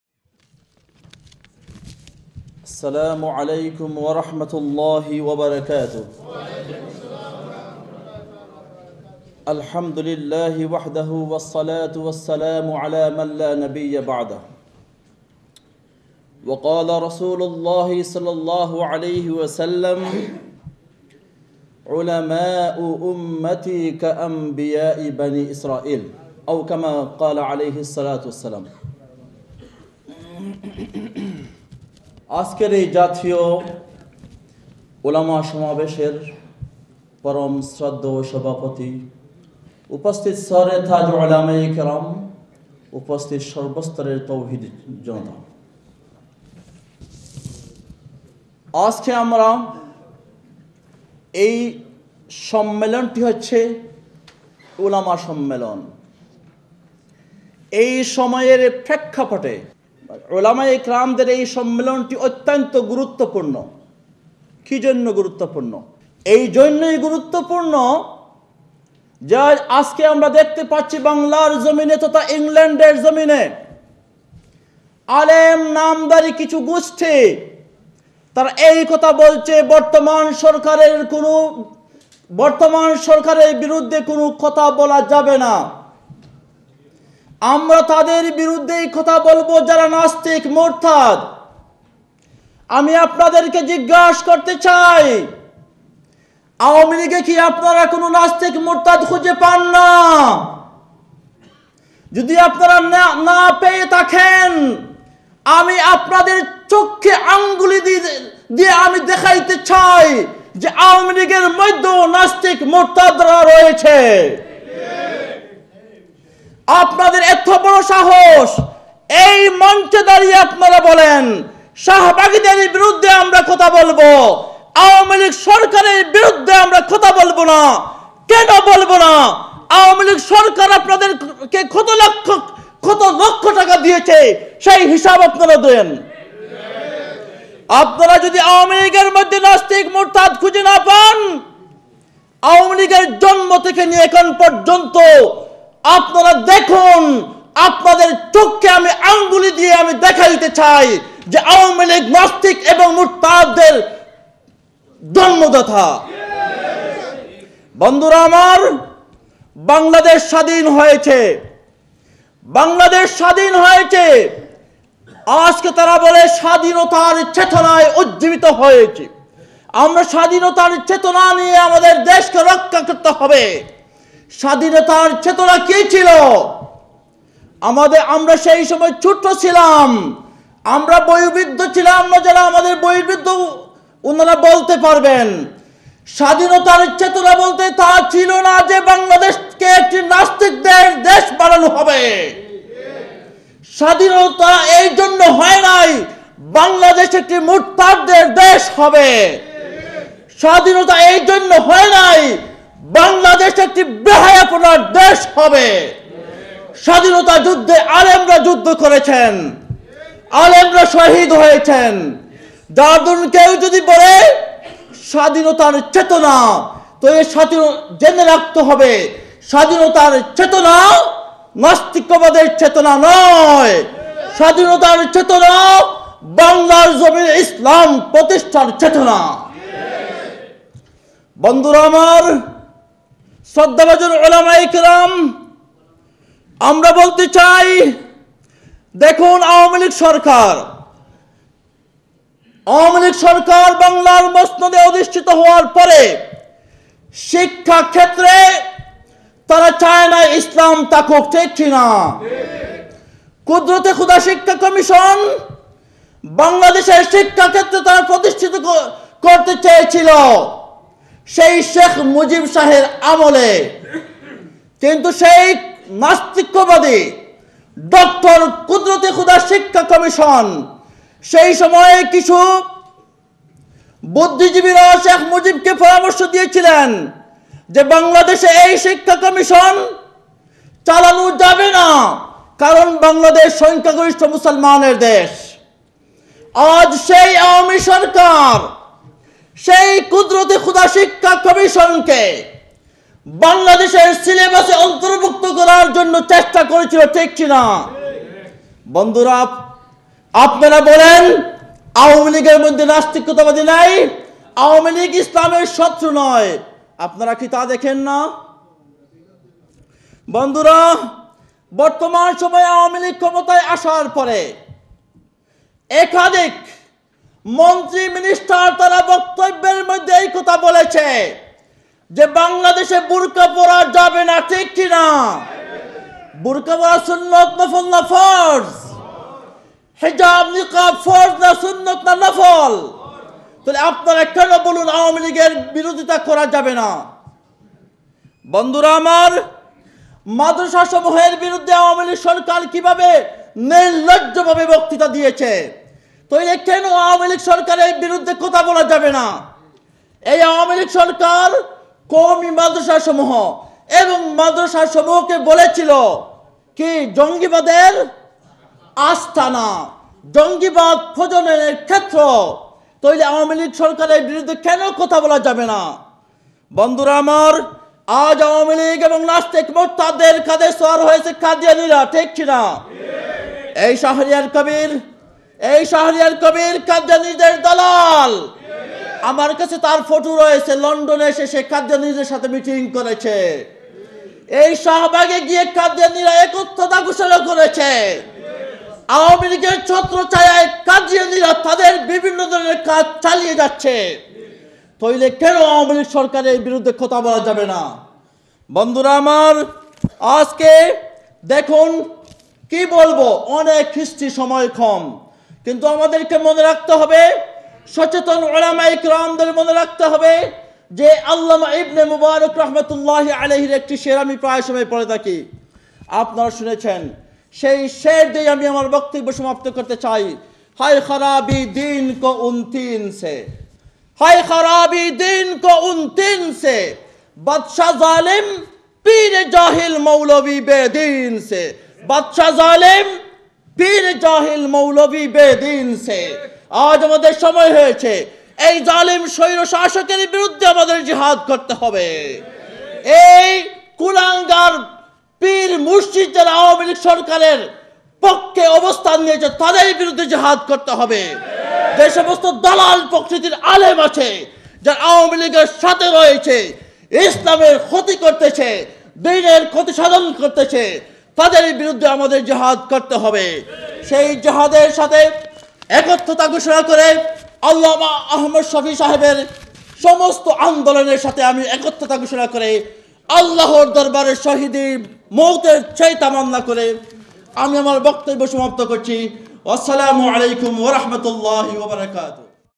Moulana Mizanul Haque, Asking WHY we Cannot Exposing Awami League
Moulana Mizanul Haque
Khilafat Majlis UK
Organised by : BANGLADESHI MUSLIMS UK
(All Party Ulama Alliance. UK)
Location: Water Lily  on 25th March 2013